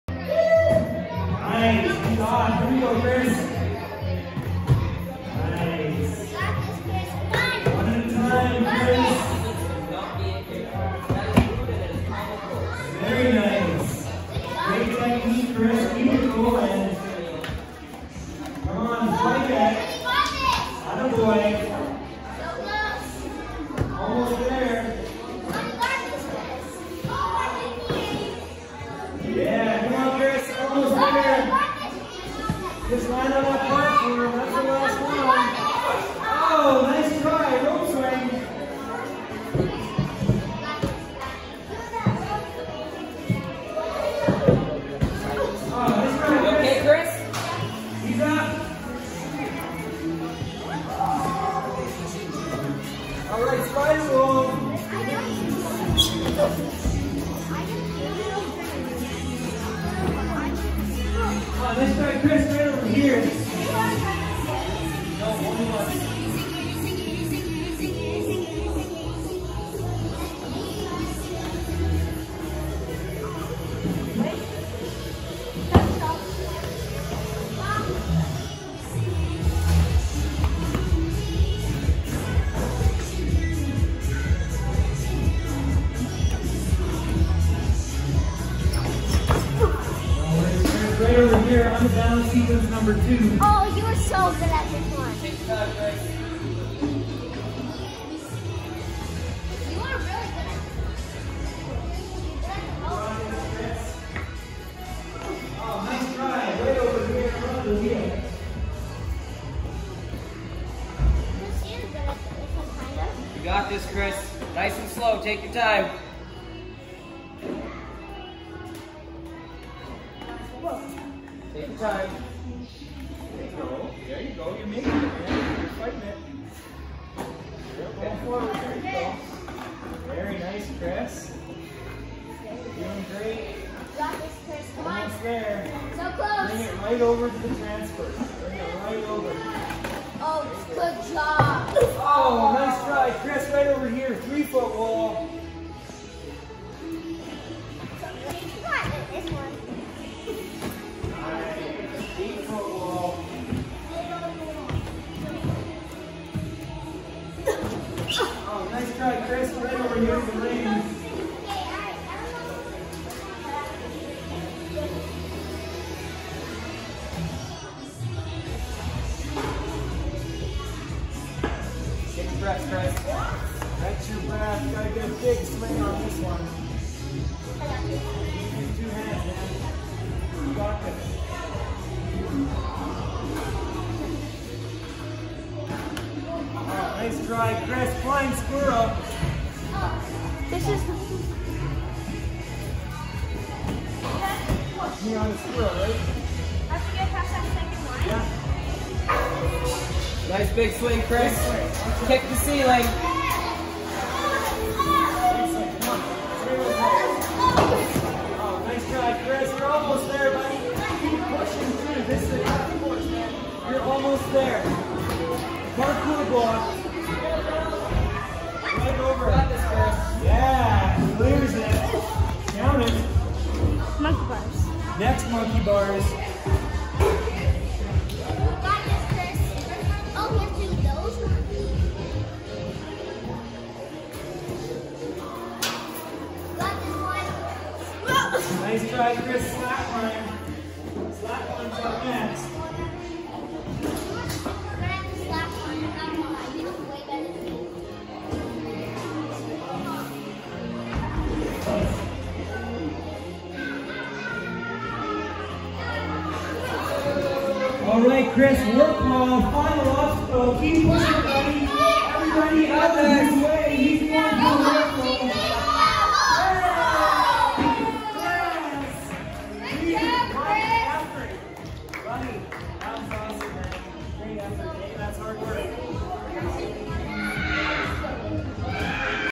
Chris' Run - Fitset Ninja Summer Camp 2019 Week 4
Check out Chris' awesome run at the end of week competition for Fitset Ninja's fourth week of summer camps!